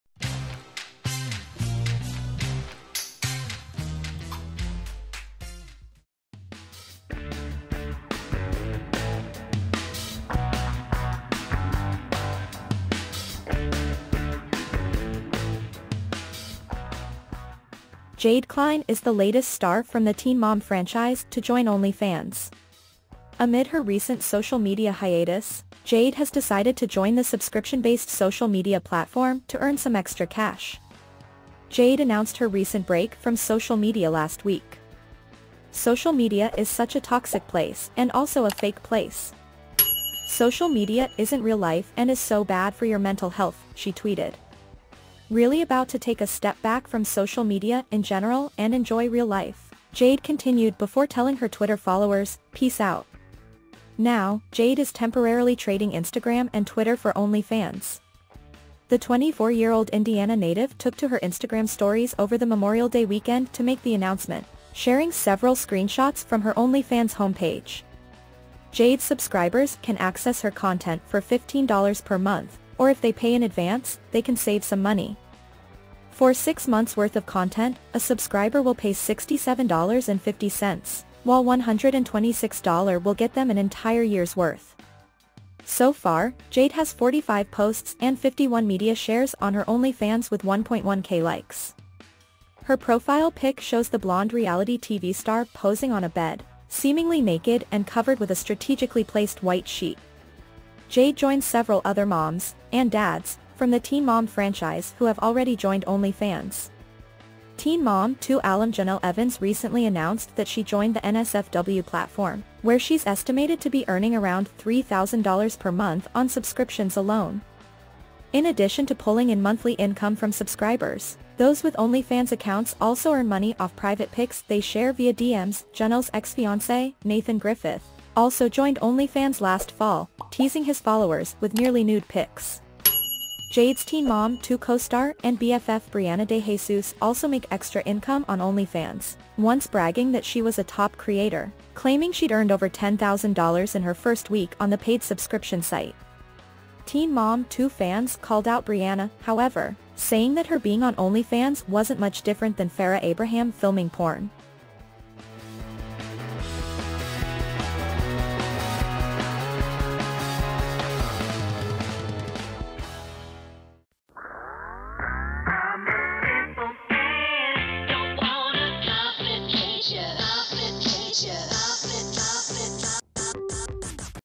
NEED EXTRA CASH!!! 'Teen Mom 2' Jade Cline Joins OnlyFans
Jade Cline is the latest star from the Teen Mom franchise to join OnlyFans.
Amid her recent social media hiatus, Jade has decided to join the subscription-based social media platform to earn some extra cash.